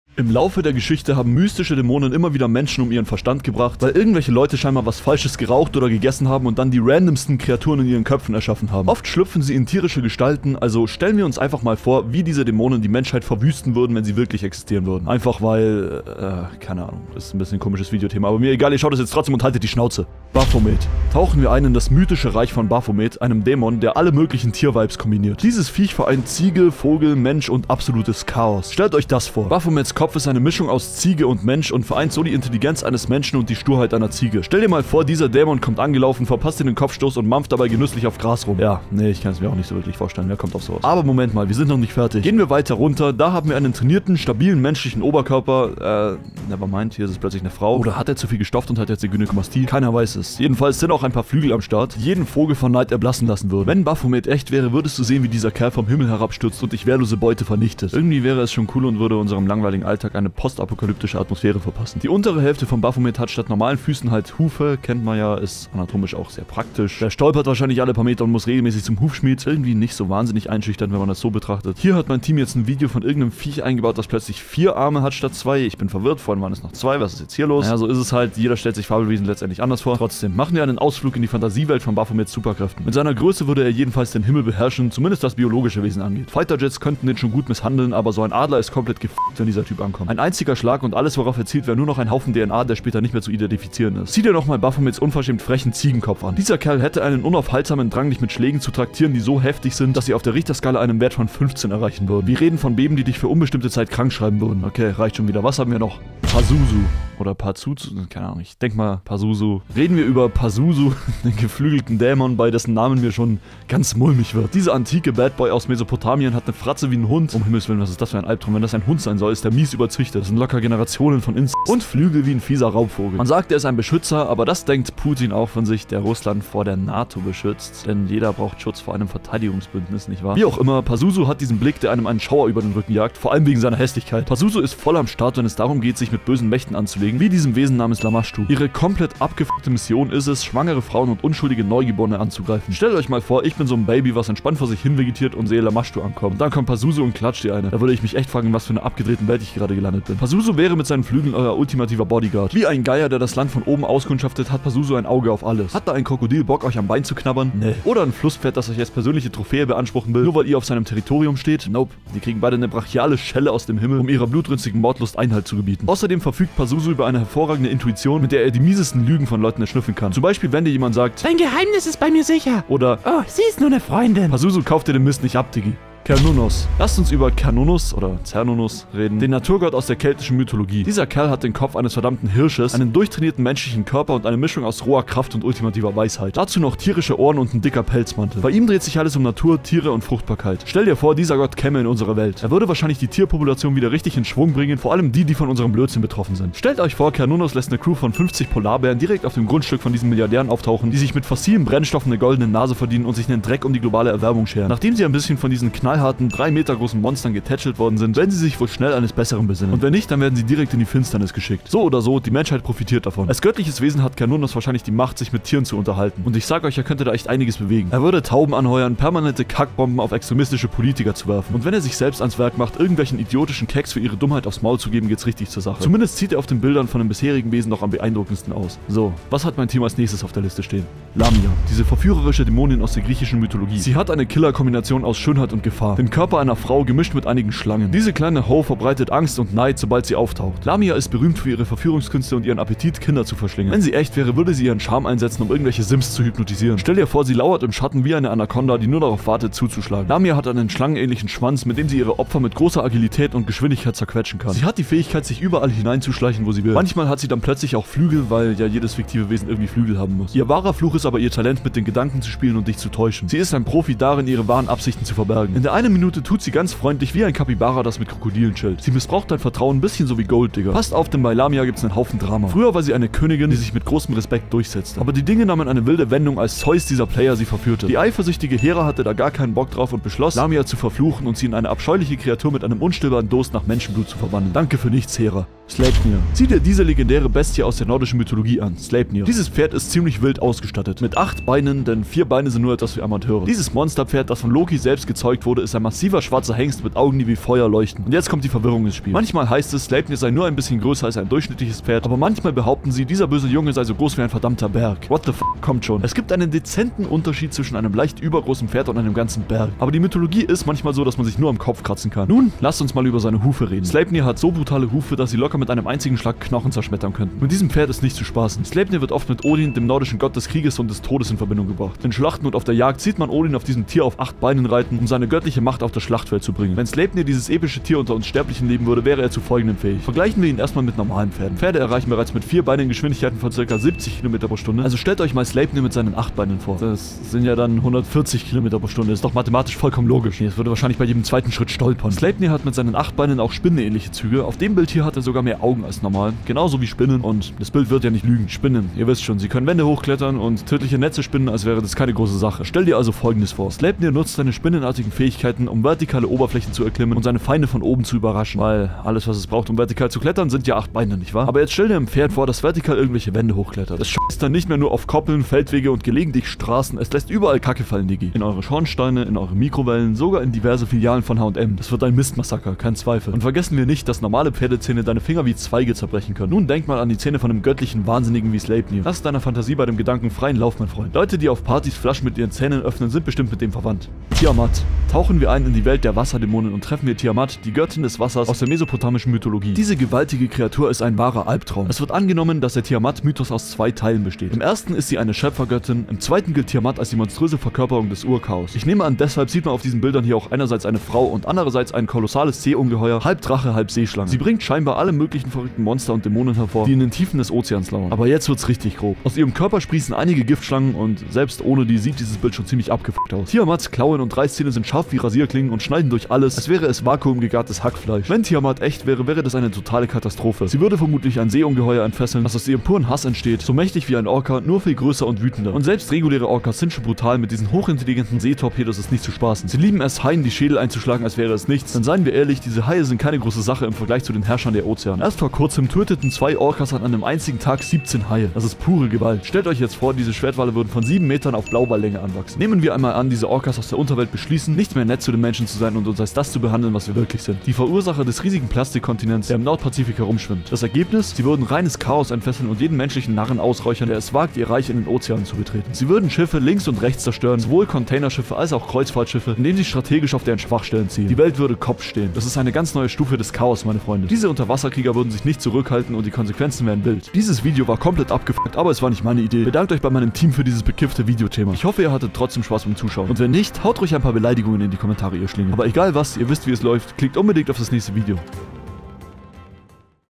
Tierische DÄMONEN der Mythologie
00:00 Intro
00:20 Baphomet
01:45 Pazuzu
02:55 Cernunnos
03:50 Lamia
04:45 Sleipnir
06:24 Tiamat